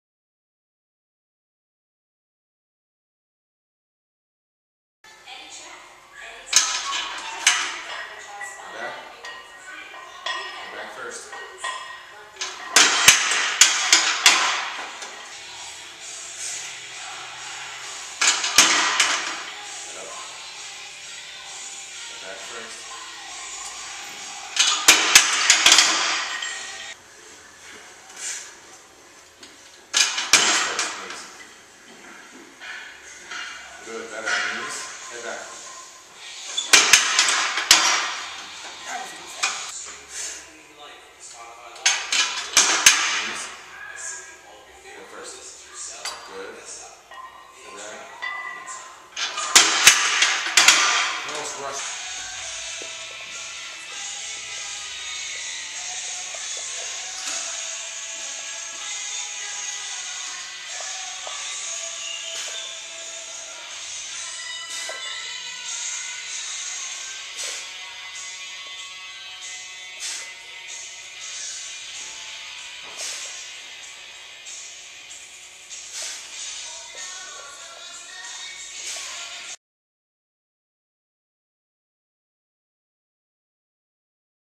Dynamic Squats with spider bar
Some light dynamic squats with the spider bar at the Elitefts compound.